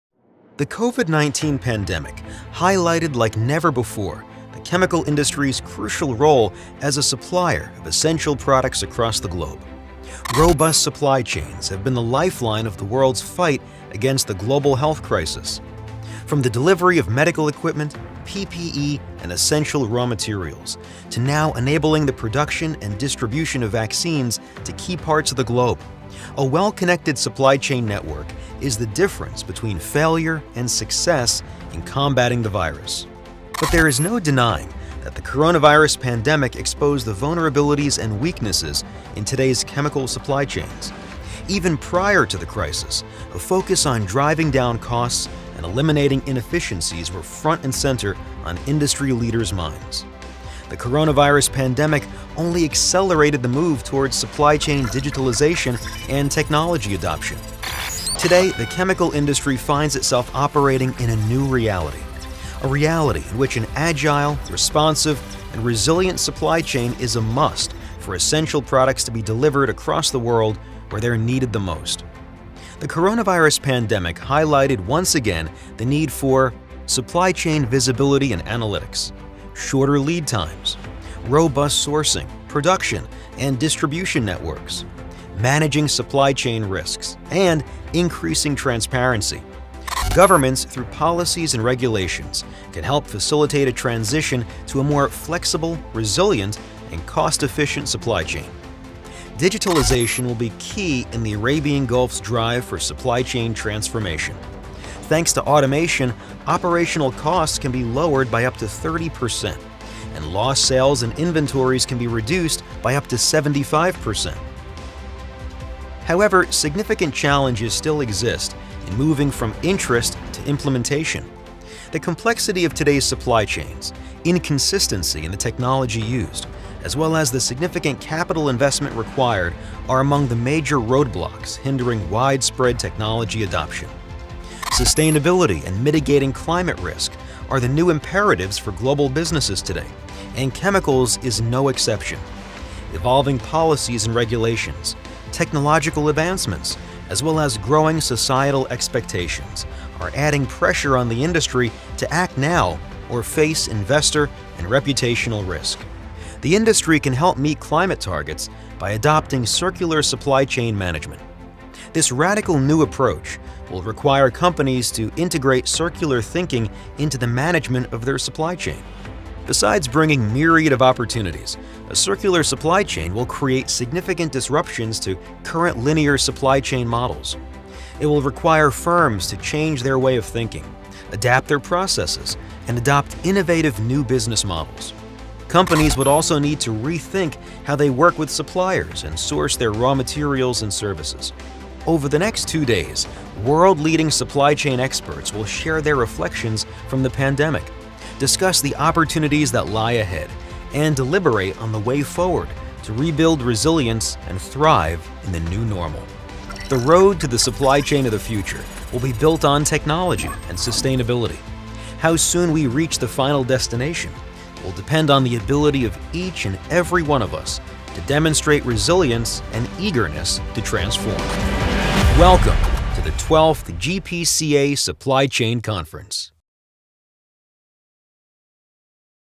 12th GPCA Supply Chain Conference curtain raiser video
Now in its 12th year, the GPCA Supply Chain Conference will discuss the challenges facing global and regional supply chains, the role of sustainability and digitalization in enabling a successful future and the lessons learned from the pandemic.